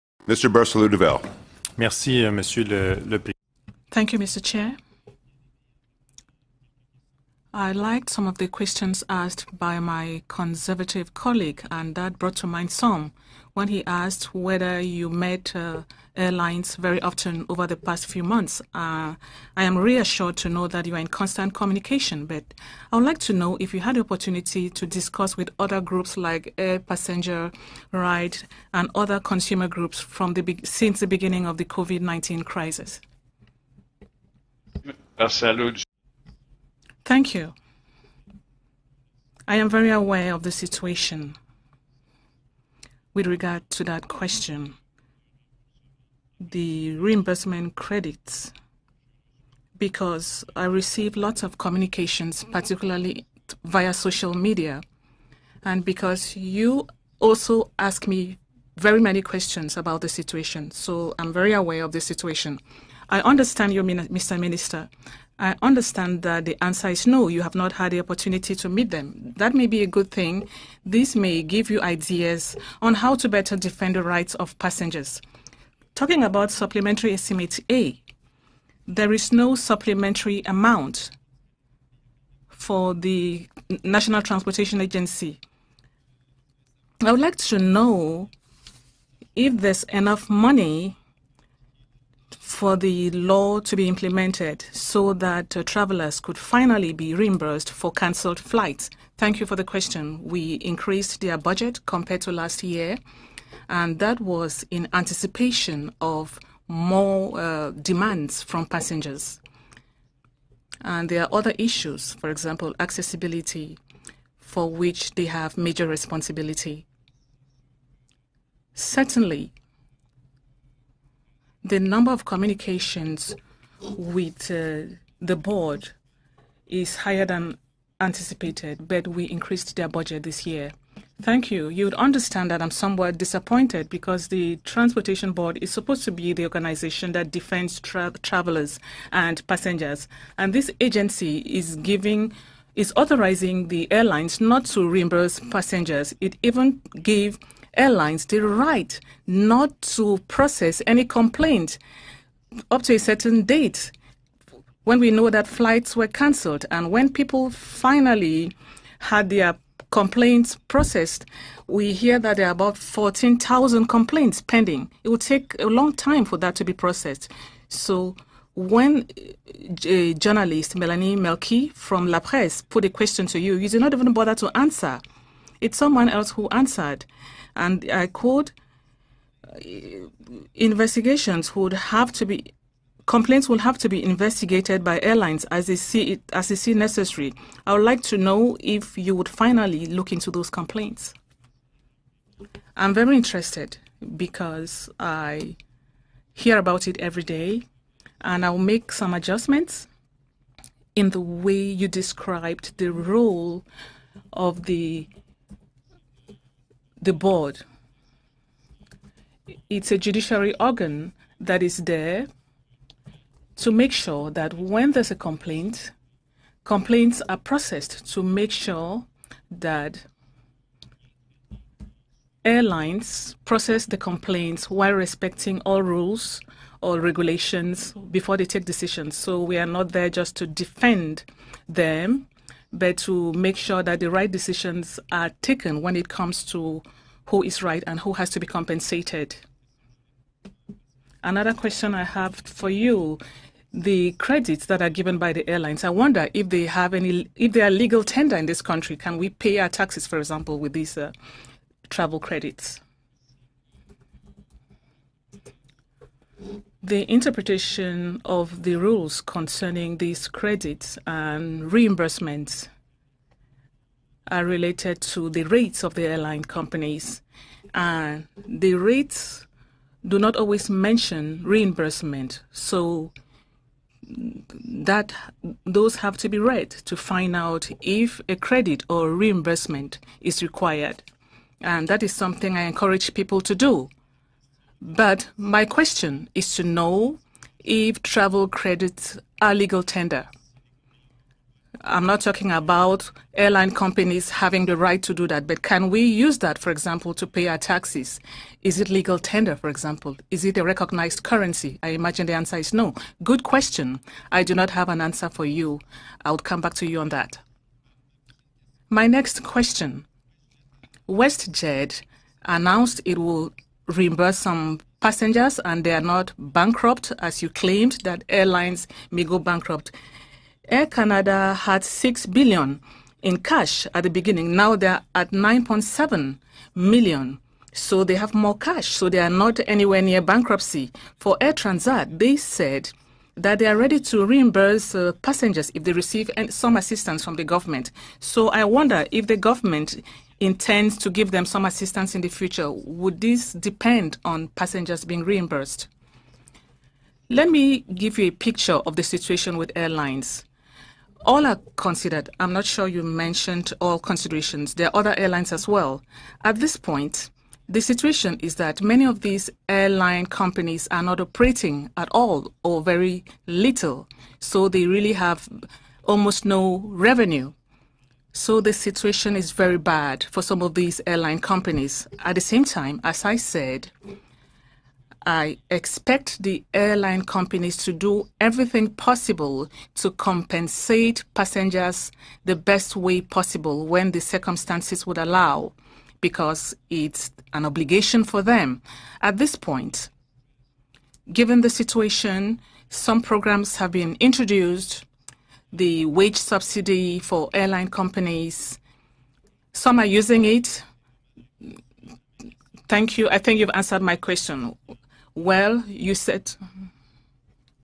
MP Xavier Barsalou-Duval questions Transport Minister Marc Garneau at TRAN (June 16, 2020)
* Did the Minister communicate with consumer groups?  0:20
* Can Canadians pay their taxes with airline vouchers? 4:45

House of Commons' Standing Committee on Transport, Infrastructure and Communities, Meeting No. 7 (June 16, 2020).